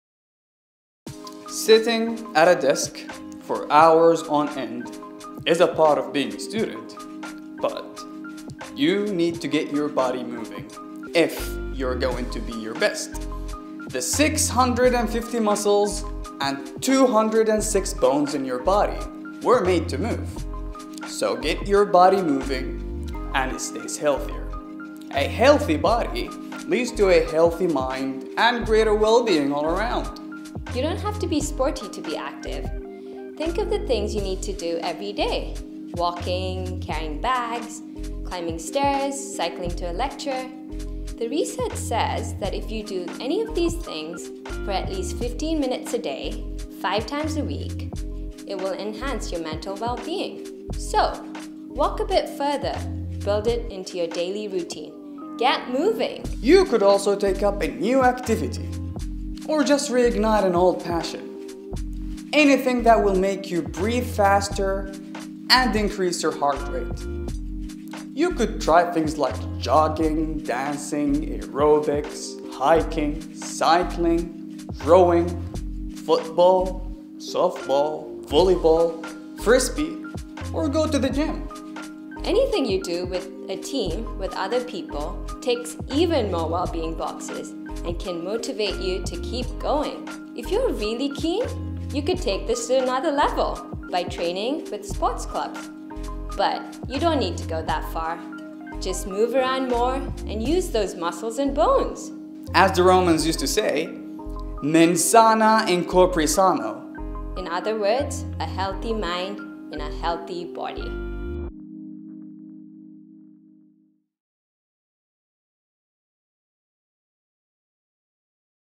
Be Active - Friends International Guide to Wellbeing (2)
With World Mental Health Day 2022 coming up on Monday, the 10th of October, we are releasing a series of 'Five Ways to Wellbeing' on YouTube and our social media for all our international friends and staff! Please share this wellbeing series with someone who you think would benefit from watching them!

Facebook, Twitter, Instagram: @friendsint

*Please note that this series was created during the reign of Her Majesty the Queen Elizabeth II.